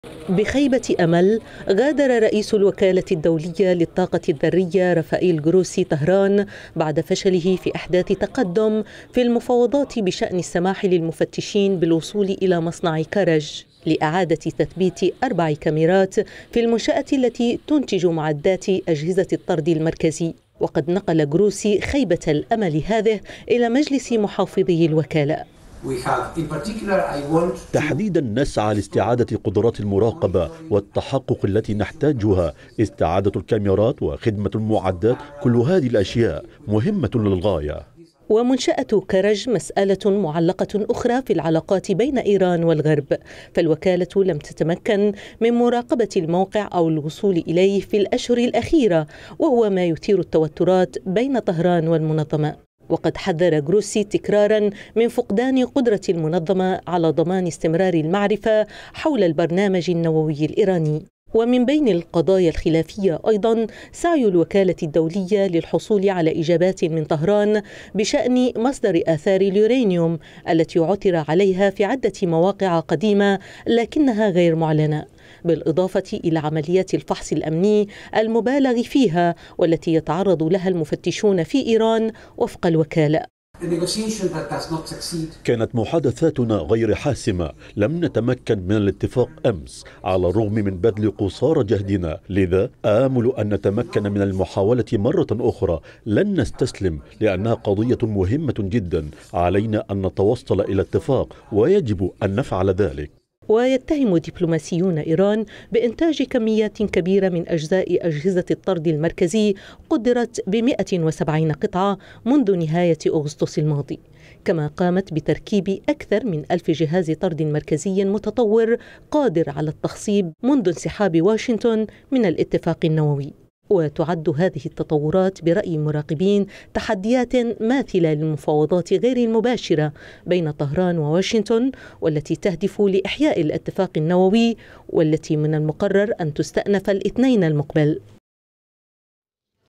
غروسي: فشلنا في اتفاق يسمح لمفتشينا بالوصول إلى "كرج"
هذا وأعلن المدير العام للوكالة الدولية للطاقة الذرية رافائيل غروسي أن المحادثات التي أجراها في طهران بشأن وصول مفتشي الوكالة إلى المنشآت النووية لم تحقق نتائج. مؤكداً أن هناك مسائل لا تزال عالقة مع طهران ويجب حلها.
تطور يأتي ليلقي بظلاله على المحادثات النووية غير المباشرة المقررة الإثنين المقبل من أجل إحياء الاتفاق النووي.